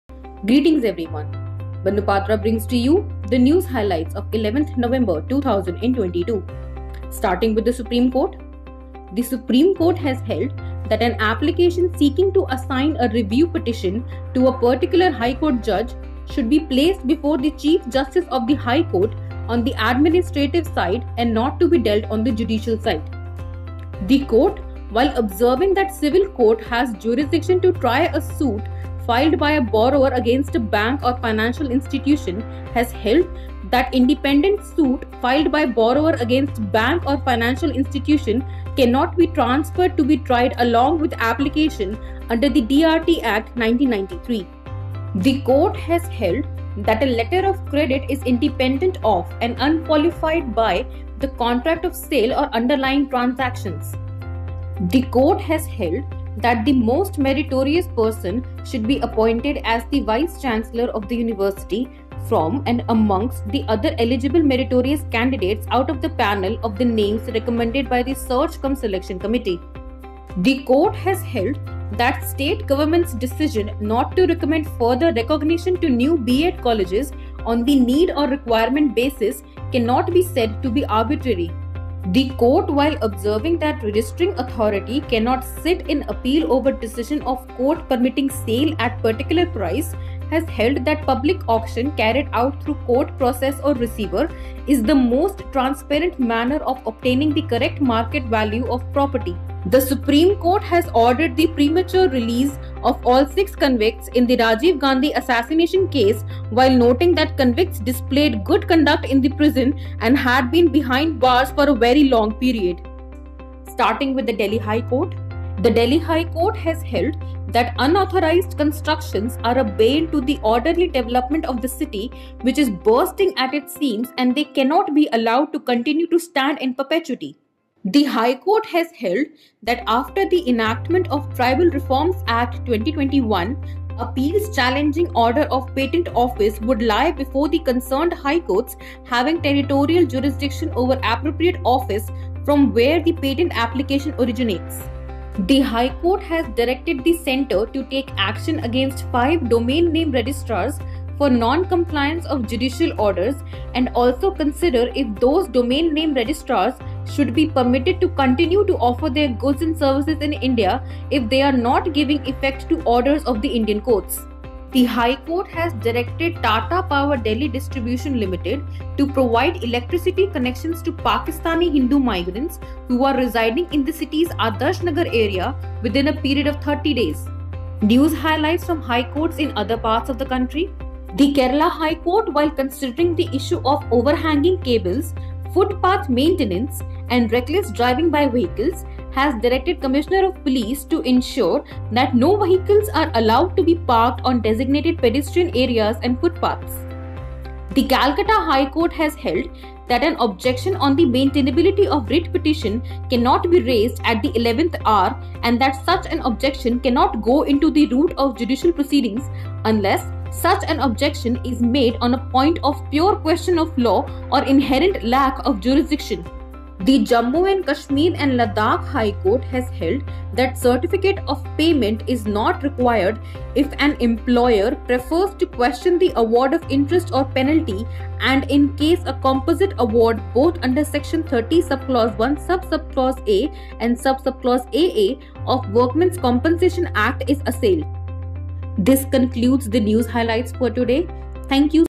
SC Orders Premature Release of All Convicts in Rajiv Gandhi Assassination Case | Manu Podcast
Video Producer : Mayank Aswal